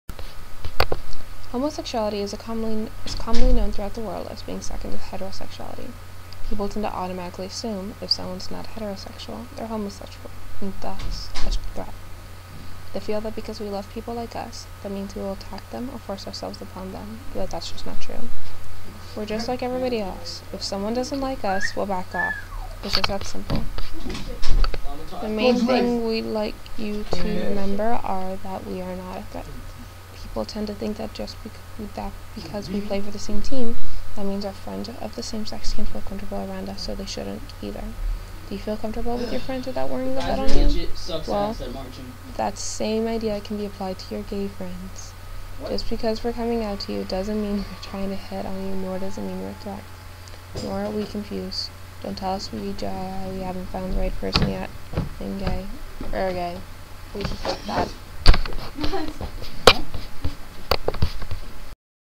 homosexuality
SORRY FOR THE LOW SOUND!
So this is what I did for my senior project. This is just the homosexual piece of the project. When it's all done, there will be several speakers on several different sexualities as well as transgender issues. There will be more videos to come. Also, everyone in these videos are volunteers.